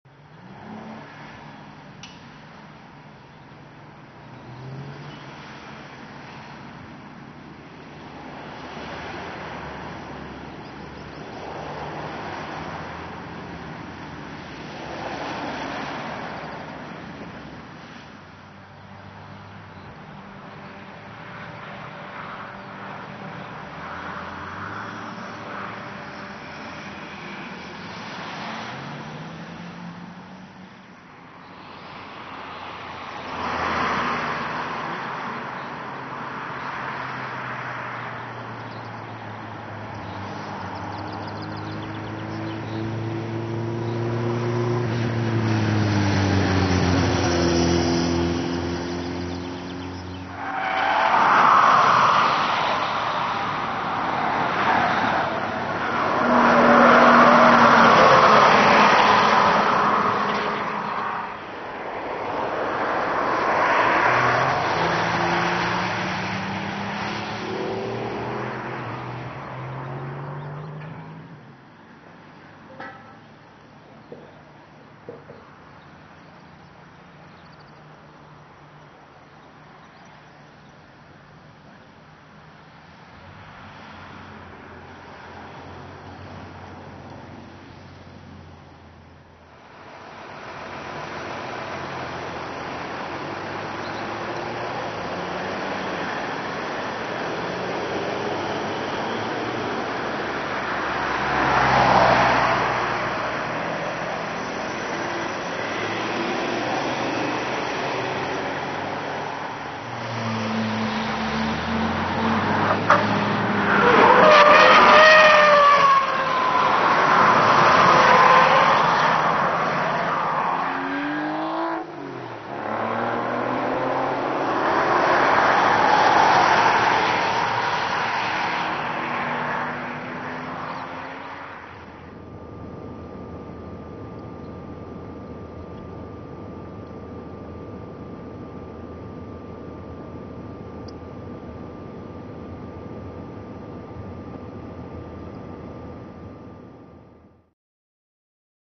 Mercedes GLK Spy Video
The upcoming Mercedes GLK compact SUV caught testing on and around the Nurburgring.